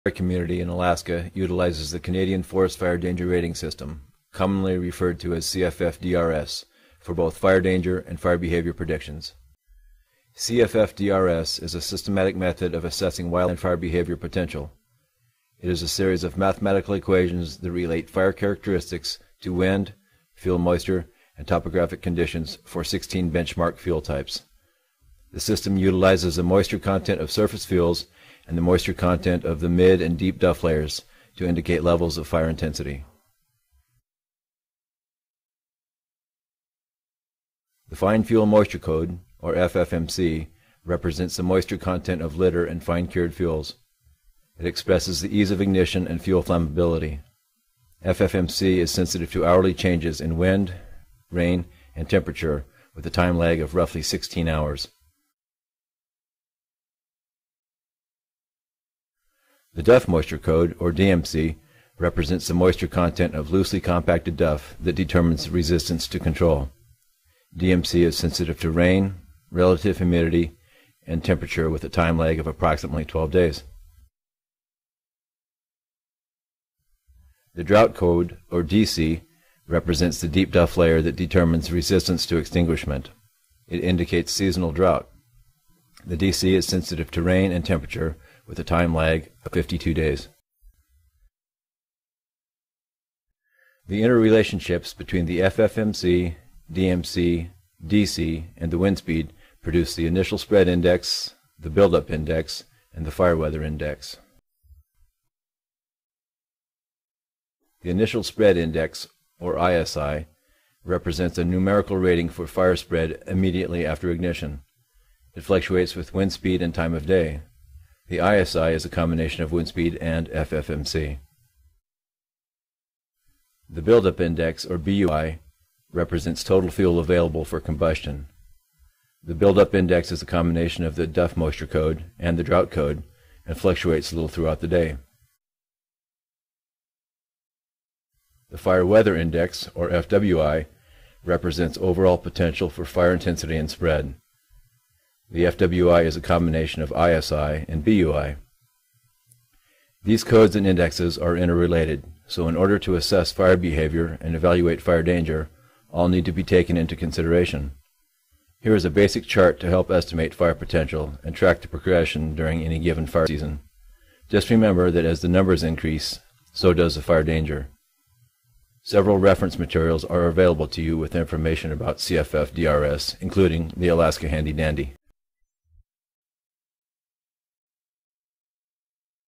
Alaska Fire Orientation - Part 06: Canadian Forest Fire Danger Rating System
This is Part 6 of the Alaska Fire Orientation video, produced by BLM Alaska Fire Service and State of Alaska Department of Natural Resources, Division of Forestry for incoming fire fighting resources.

The Alaska Fire Orientation video is available on YouTube as a preparation and reference tool. If you are traveling to Alaska on assignment, you will be required to watch the video on arrival.